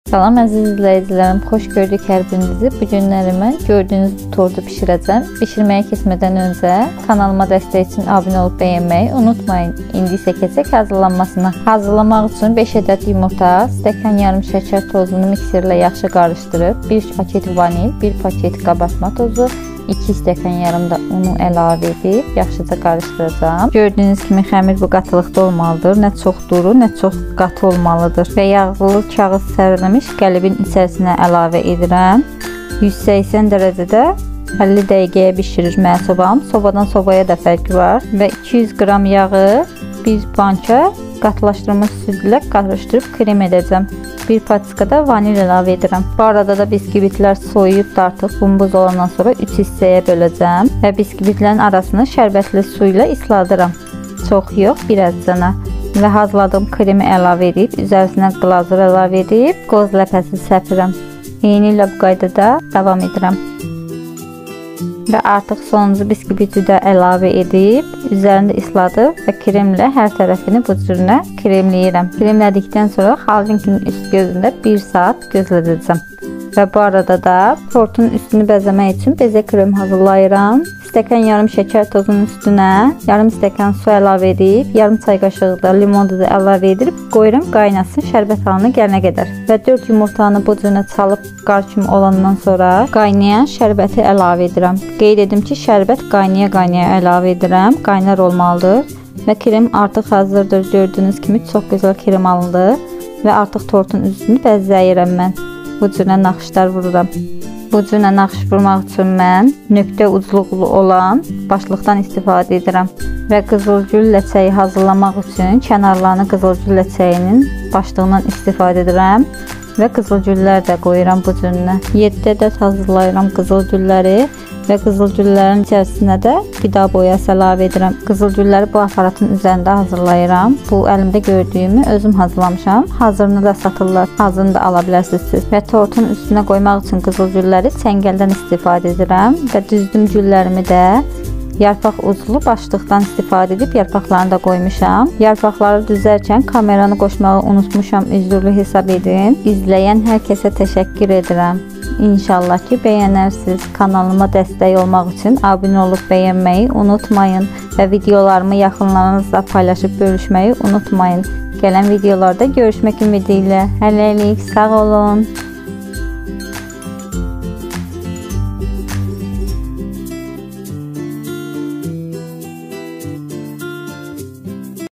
Qızıl gül bəzəkli tort hərkəsin hazırlaya biləcəyi çoxx asandır
Resepti :📖
Xəmiri
5. Ədəd yumurta
1.5 Stəkan şəkər tozu
1.Paket vanil
1.Paket kabartma tozu
2.5 Stəkan un

Yağlı kremi
200. Qram kərə yağı
1.Banka qatlaşdırılmış süd
1.Paket vanil

Beze kremi
4. Ədəd yumurta ağı
1.5 Stəkan şəkər tozu
0.5 Stəkan su
0.5 Çay qaşığı limon duzu